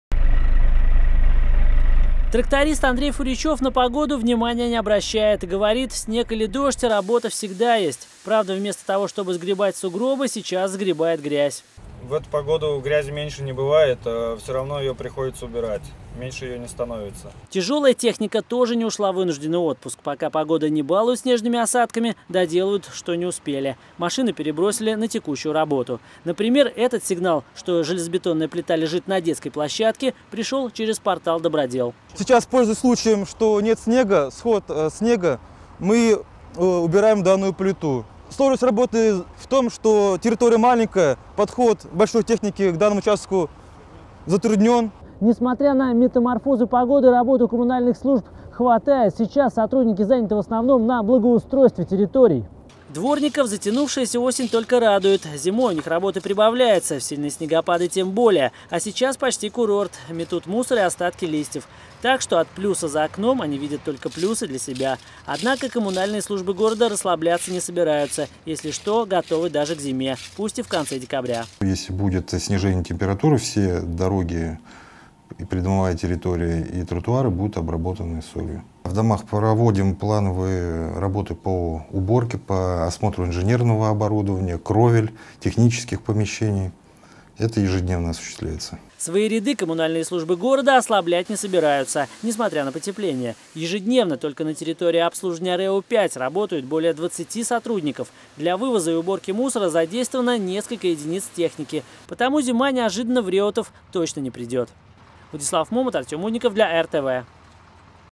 ЧТО КОММУНАЛЬНЫЕ СЛУЖБЫ ДЕЛАЮТ ВО ВРЕМЯ ПОТЕПЛЕНИЯ 22.12.15
Погода бьет все температурные рекорды и удивляет совсем не характерными для конца декабря явлениями. Например, сегодня практически весь день шел вполне весенний день. Мы решили выяснить, а чем занимаются во время потепления коммунальщики, которые вроде уже приготовились к зиме.